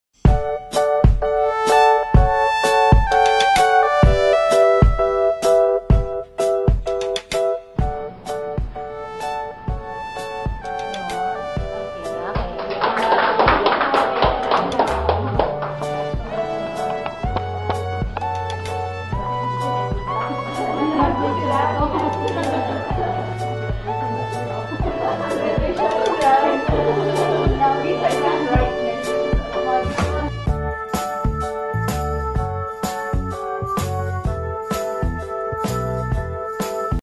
Let’s Applaud & Celebrate With Lory Jane Librando - Spouse Visa
Read, Watch and Discover, as our Satisfied Filipino Applicants & Australian Sponsors share their stories about the remarkable migration service they experienced during their journey from the Philippines.